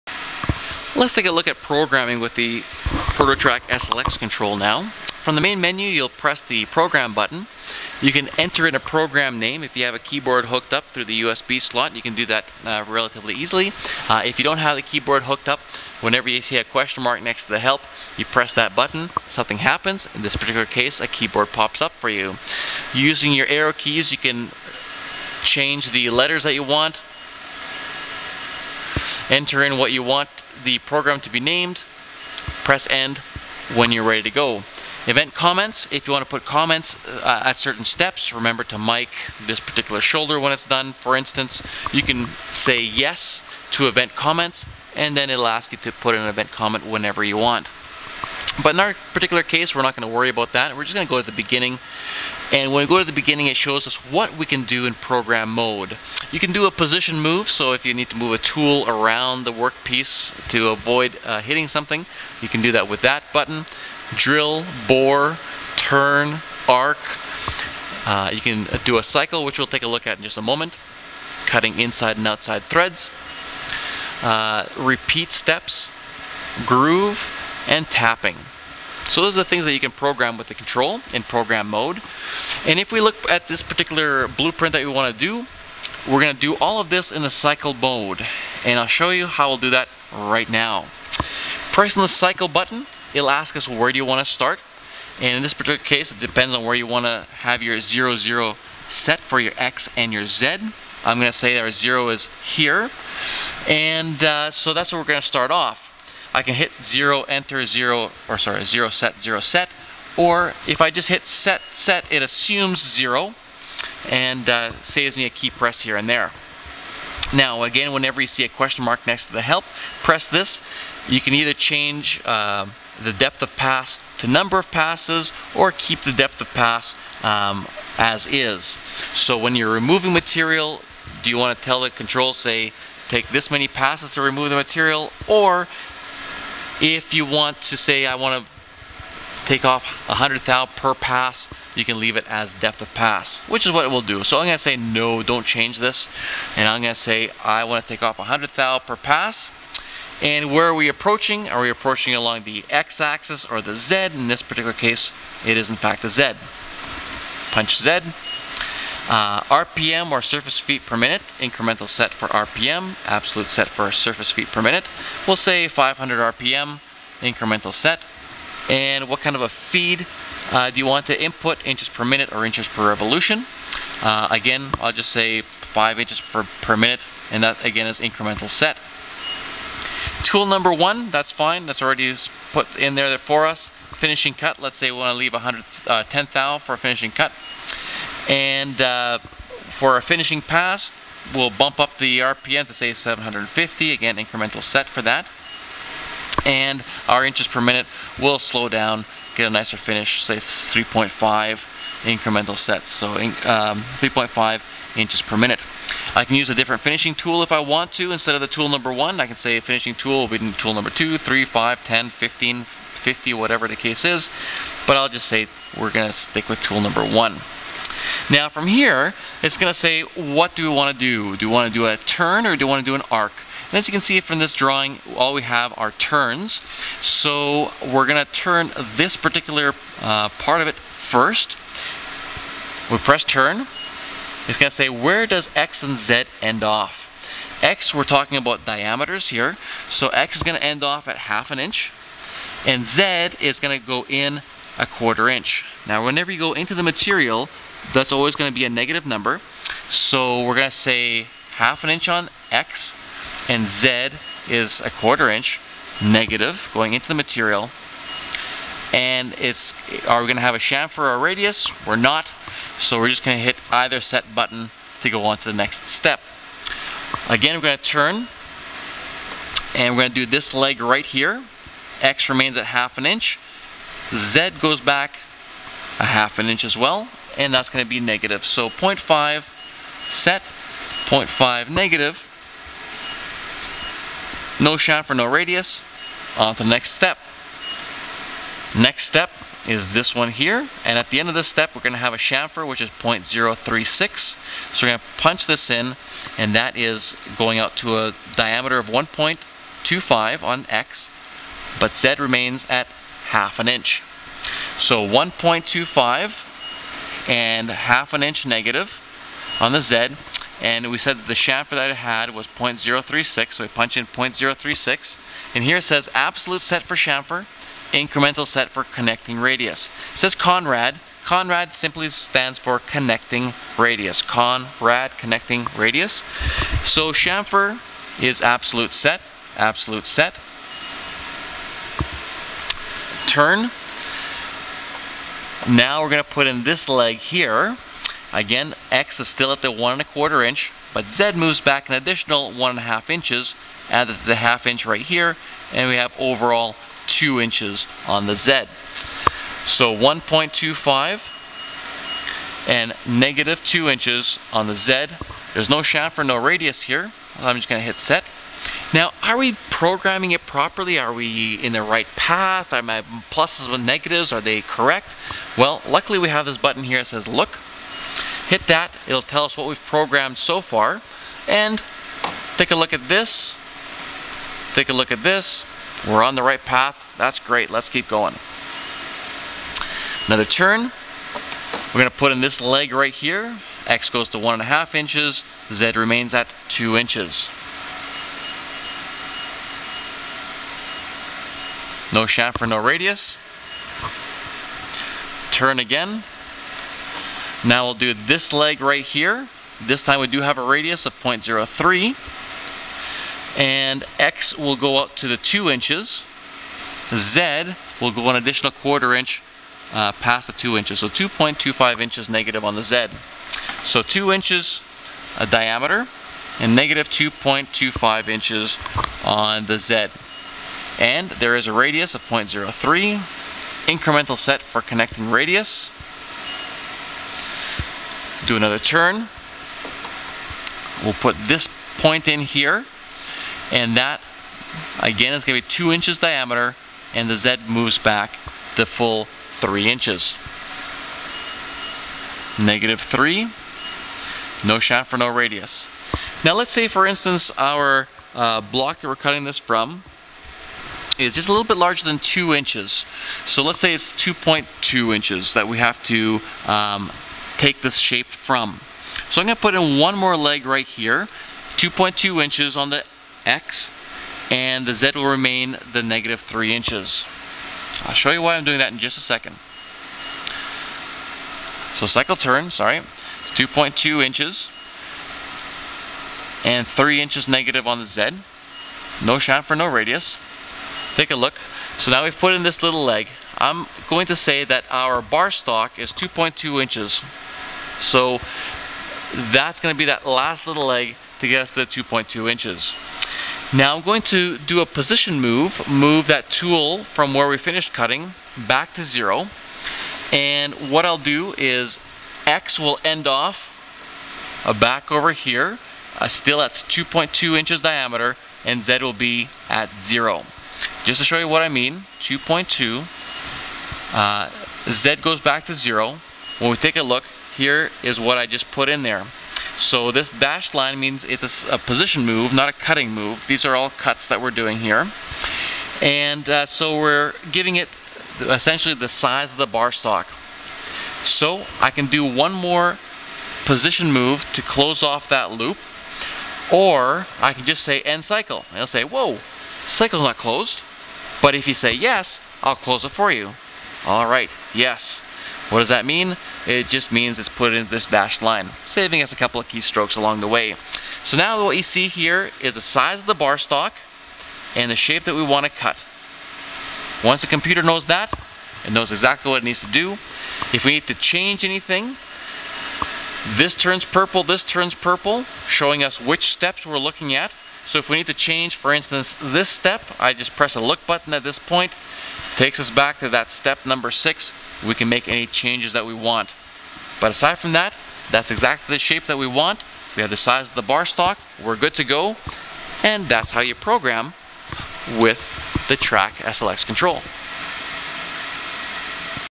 TRAK SLX 2-ax CNC control 2/3
Overview of the TRAK CNC SLX lathe control - Programming mode